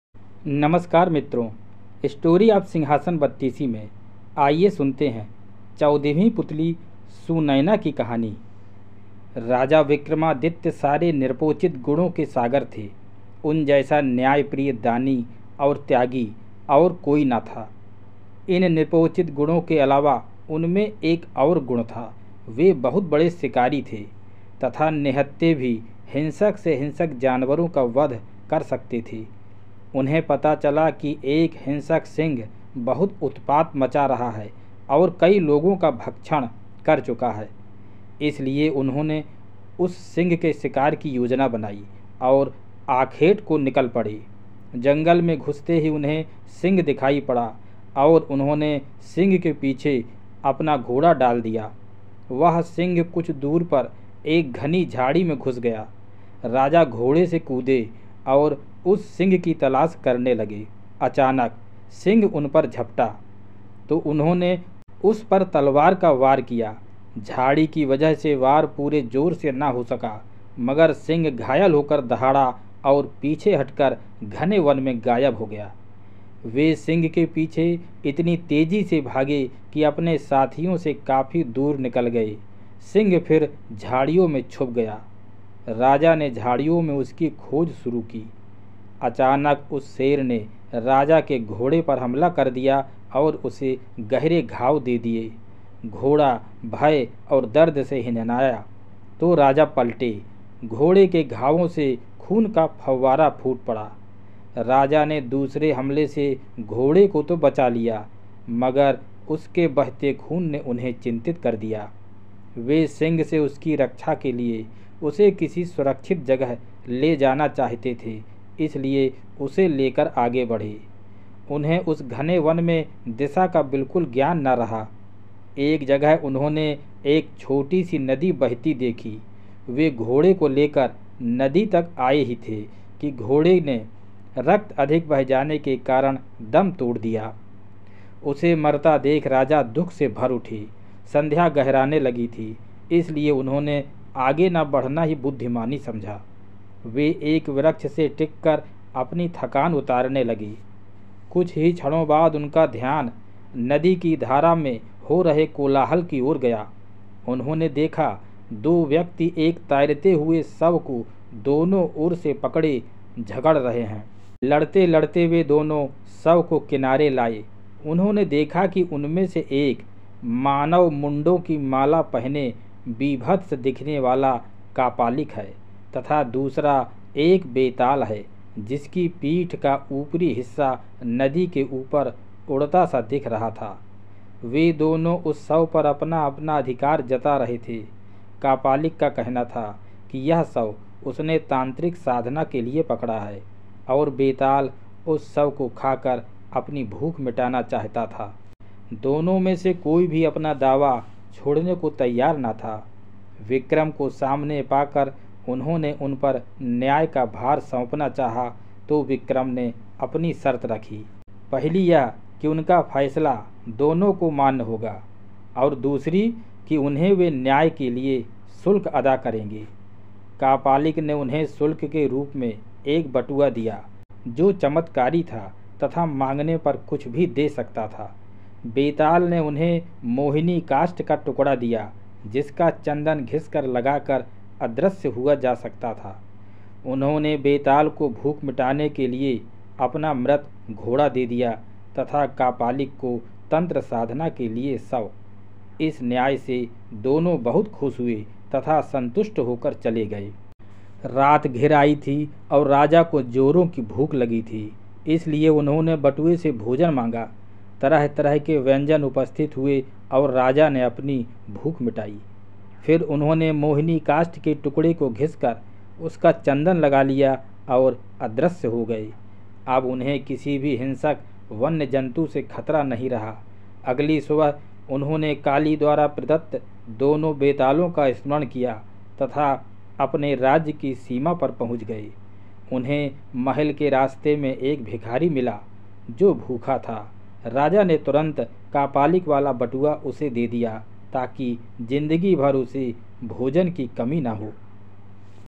चौदहवीं पुतली सुनयना/सिंहासन बत्तीसी Sunayana (Hindi Story)
सुनयना : हिंसक सिंह का शिकार : सिंहासन बत्तीसीaudiobook
Sunayana (Hindi Story) : Sinhasan Battisi#rochakhindikahaniya
चौदहवीं पुतली - सुनयना ने जो कथा की वह इस प्रकार है-

राजा विक्रमादित्य सारे नृपोचित गुणों के सागर थे। उन जैसा न्यायप्रिय, दानी और त्यागी और कोई न था। इन नृपोचित गुणों के अलावा उनमें एक और गुण था। वे बहुत बड़े शिकारी थे तथा निहत्थे भी हिंसक से हिंसक जानवरों का वध कर सकते थे। उन्हें पता चला कि एक हिंसक सिंह बहुत उत्पात मचा रहा है और कई लोगों का भक्षण कर चुका है, इसलिए उन्होंने उस सिंह के शिकार की योजना बनाई और आखेट को निकल पड़े। जंगल में घुसते ही उन्हें सिंह दिखाई पड़ा और उन्होंने सिंह के पीछे अपना घोड़ा डाल दिया। वह सिंह कुछ दूर पर एक घनी झाड़ी में घुस गया। राजा घोड़े से कूदे और उस सिंह की तलाश करने लगे। अचानक सिंह उन पर झपटा, तो उन्होंने उस पर तलवार का वार किया। झाड़ी की वजह से वार पूरे ज़ोर से नहीं हो सका, मगर सिंह घायल होकर दहाड़ा और पीछे हटकर घने वन में गायब हो गया।

वे सिंह के पीछे इतनी तेज़ी से भागे कि अपने साथियों से काफी दूर निकल गए। सिंह फिर झाड़ियों में छुप गया। राजा ने झाड़ियों में उसकी खोज शुरु की। अचानक उस शेर ने राजा के घोड़े पर हमला कर दिया और उसे गहरे घाव दे दिए। घोड़ा भय और दर्द से हिनहिनाया, तो राजा पलटे। घोड़े के घावों से खून का फव्वारा फूट पड़। राजा ने दूसरे हमले से घोड़े को तो बचा लिया, मगर उसके बहते खून ने उन्हें चिन्तित कर दिया। वे सिंह से उसकी रक्षा के लिए उसे किसी सुरक्षित जगह ले जाना चाहते थे, इसलिए उसे लेकर आगे बढ़े। उन्हें उस घने वन में दिशा का बिल्कुल ज्ञान नहीं रहा। एक जगह उन्होंने एक छोटी सी नदी बहती देखी। वे घोड़े को लेकर नदी तक आए ही थे कि घोड़े ने रक्त अधिक बह जाने के कारण दम तोड़ दिया।

उसे मरता देख राजा दुख से भर उठे। संध्या गहराने लगी थी, इसलिए उन्होंने आगे न बढ़ना ही बुद्धिमानी समझा। वे एक वृक्ष से टिककर अपनी थकान उतारने लगे। कुछ ही क्षणों बाद उनका ध्यान नदी की धारा में हो रहे कोलाहल की ओर गया उन्होंने देखा दो व्यक्ति एक तैरते हुए शब को दोनों ओर से पकड़े झगड़ रहे हैं। लड़ते-लड़ते वे दोनों शव को किनारे लाए। उन्होंने देखा कि उनमें से एक मानव मुण्डों की माला पहने बीभत्स दिखने वाला कापालिक है तथा दूसरा एक बेताल है जिसकी पीठ का ऊपरी हिस्सा नदी के ऊपर उड़ता-सा दिख रहा था। वे दोनों उस शव पर अपना-अपना अधिकार जता रहे थे। कापालिक का कहना था कि यह शव उसने तांत्रिक साधना के लिए पकड़ा है और बेताल उस शव को खाकर अपनी भूख मिटाना चाहता था।